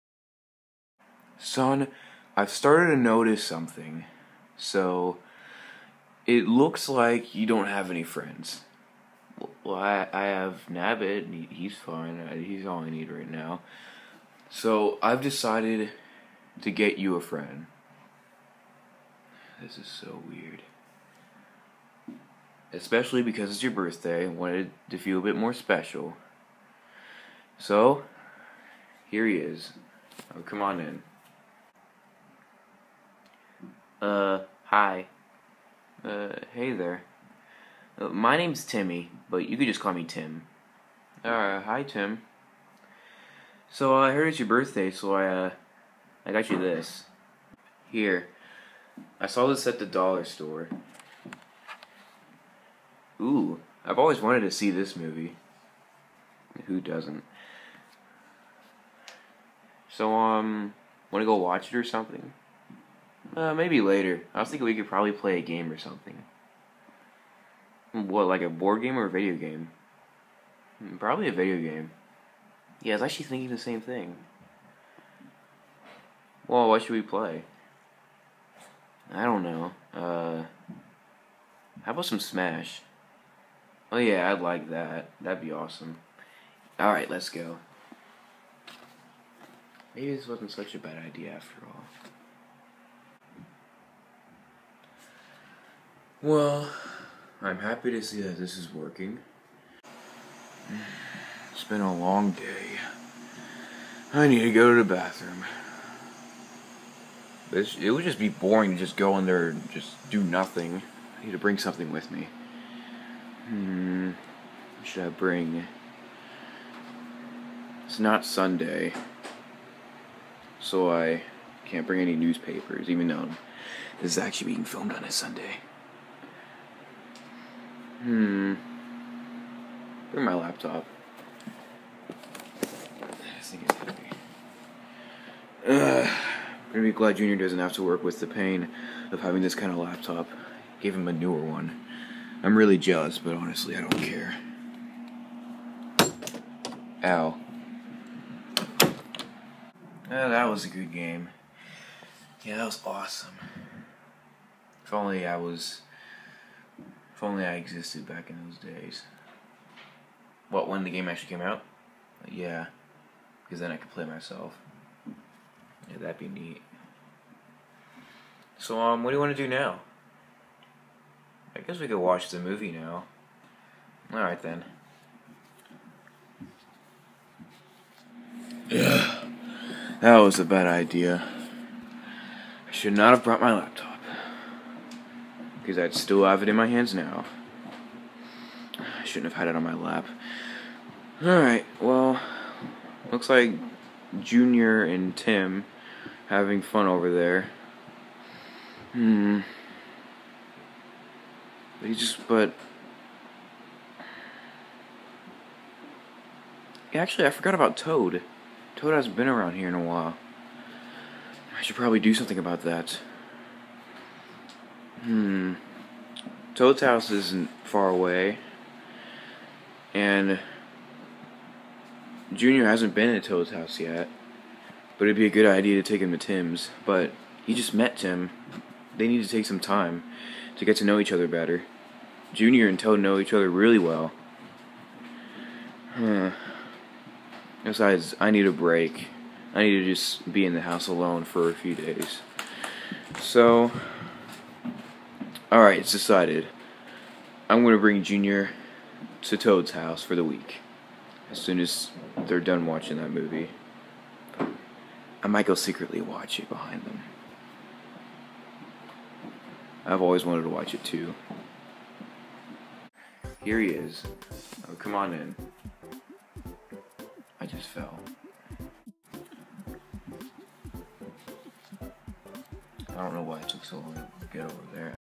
Diary of a Wimpy Koopa - Episode 7
Bowser gets Junior a new friend!